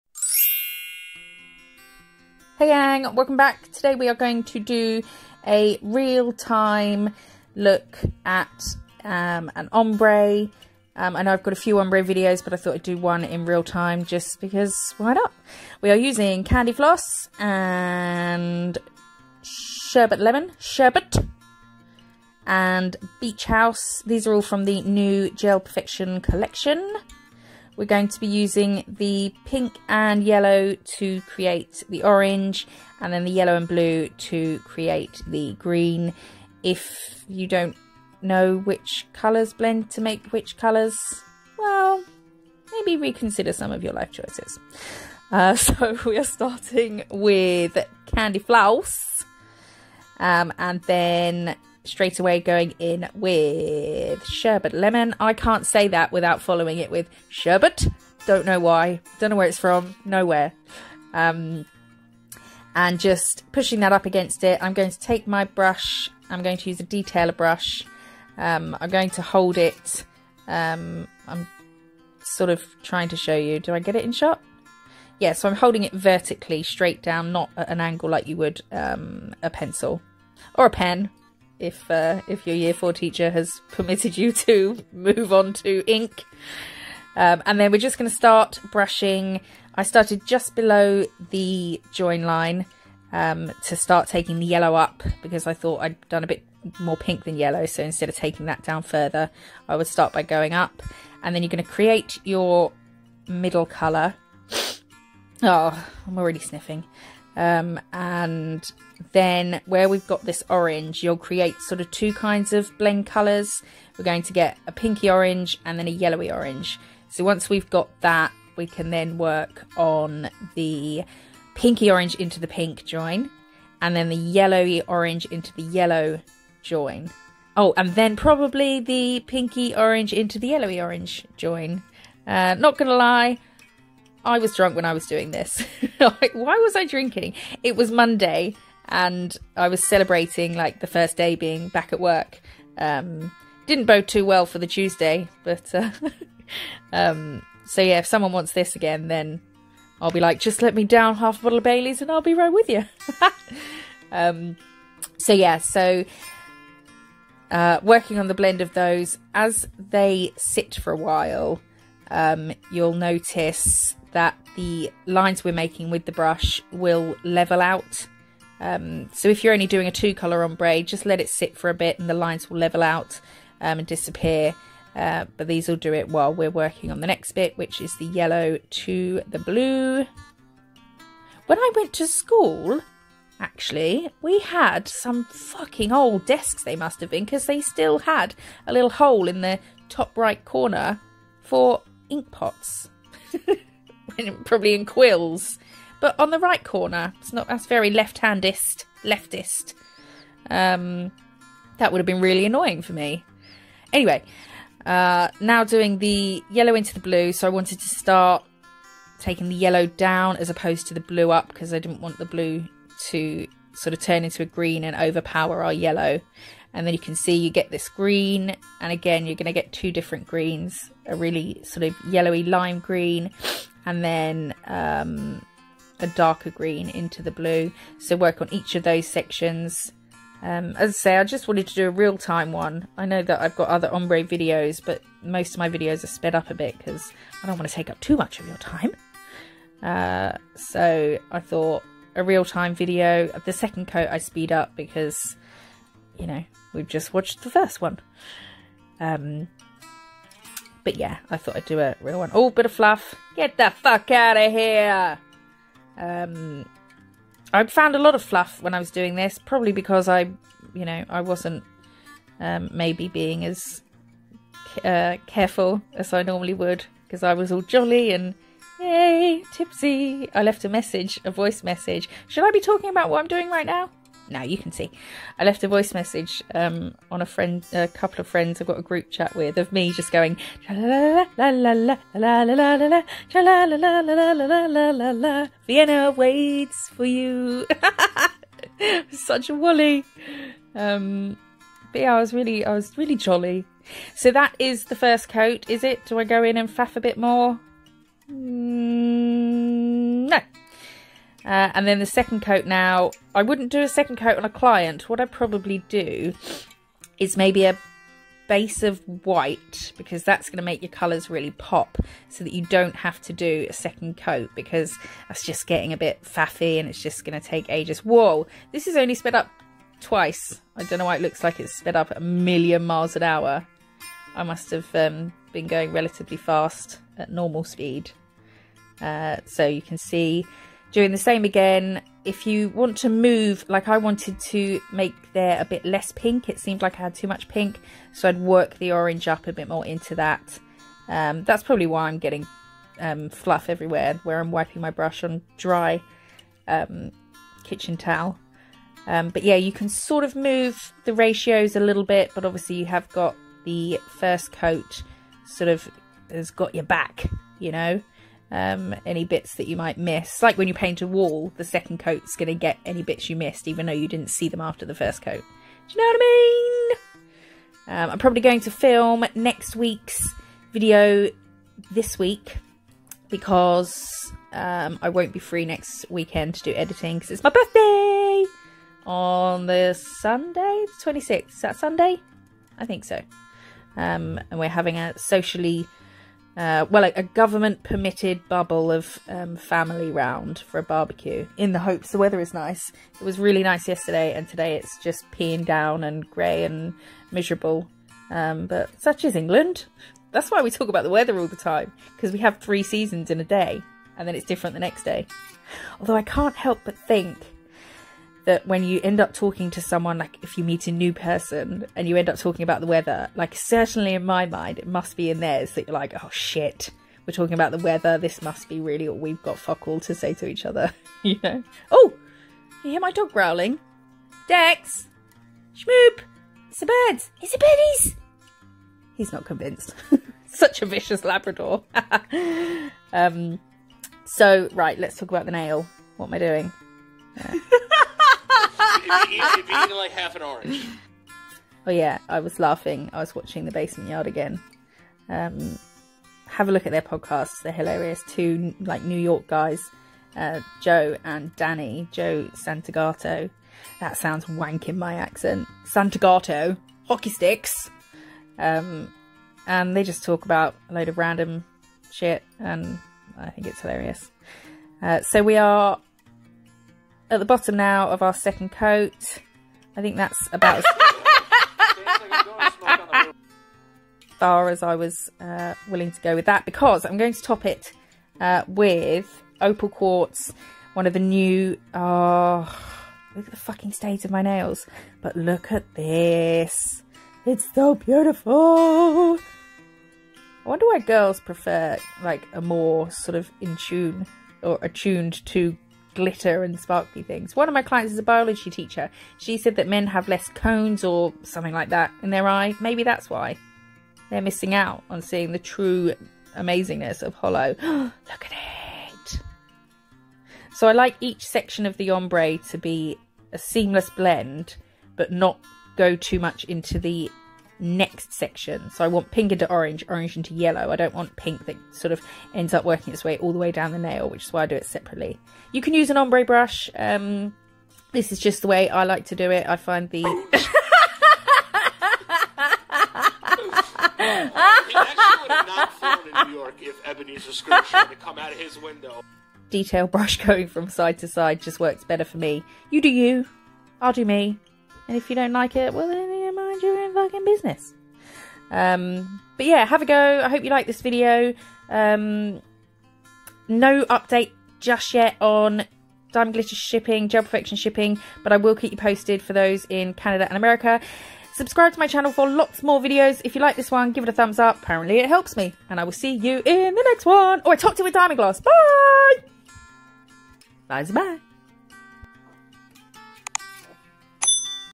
🌈 HOW TO - RAINBOW OMBRÉ | Real time | Gel polish ombre nail art tutorial
Hey gang! A closer, real time look at a gel polish ombré. Hope you like it! Products down there ⬇️

💅🏽 PRODUCTS:
Candy Floss
Sherbet Lemon
Beach Hut
Opal Quartz
Diamond Gloss